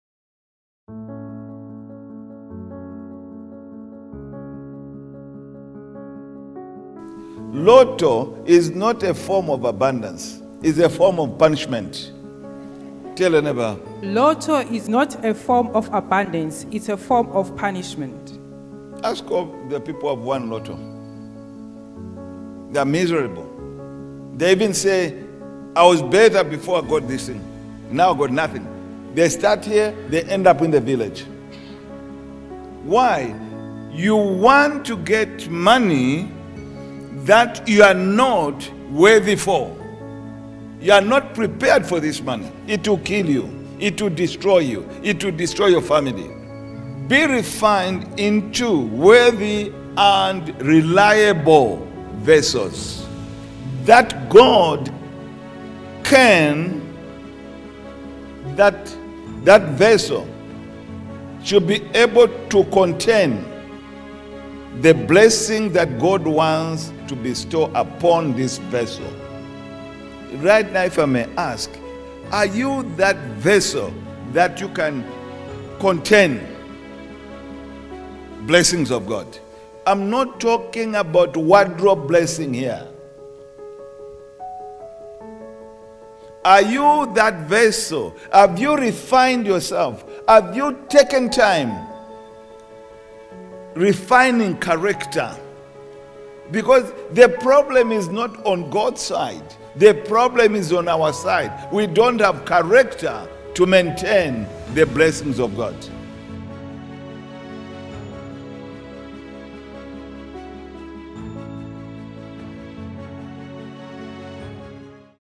LOTTO IS A FORM OF PUNISHMENT💸💸💸
We must first build ourselves. We should be vessels worthy of abundance.
How do you become a better person? Through refinement of your character traits.
As long as your character is not refined, forget about abundance.
Lotto is not a form of abundance, it is a form of punishment.
Why? You want to get the money you're not worthy of - it will destroy you and your family. Be refined into a reliable vessel, that God can pour abundance upon.